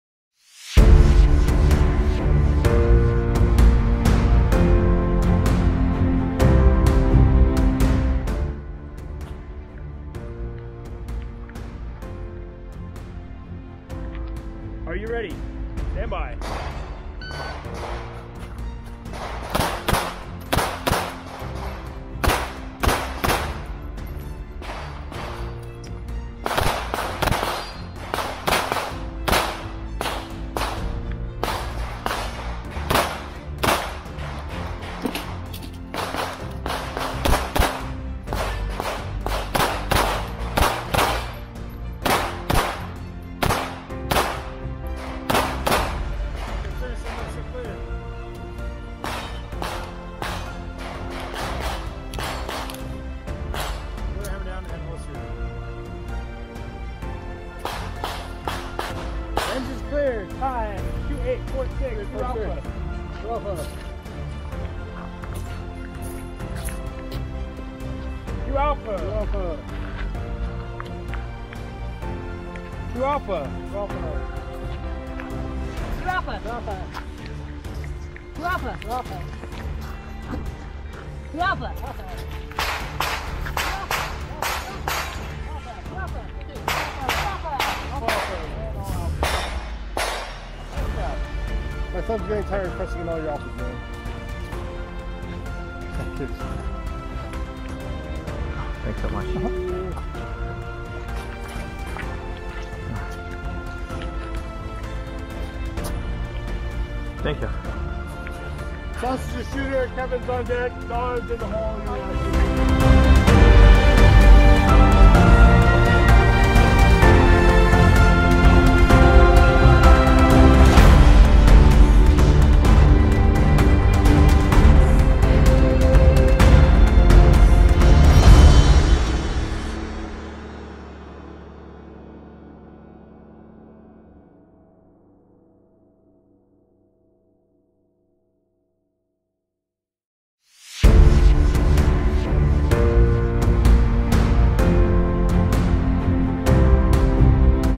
CZ Shadow2 Hybrid_All Alpha
CZ shadow 2 Hybrid is made of Shadow 2 Orange parts and Shadow 2 OR slide.
I love this and I really want to know how much I can increase the speed with this gun.
See you guys in the range! 😉👍👍🙏